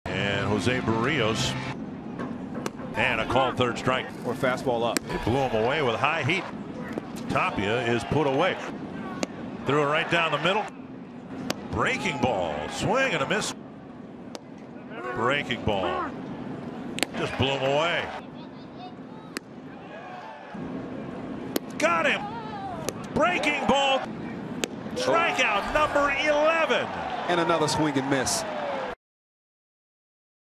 COL@MIN: Berrios strikes out 11 over 7 2/3 innings
Jose Berrios had a spectacular performance against the Rockies, striking out 11 batters and only allowing two hits over 7 2/3 innings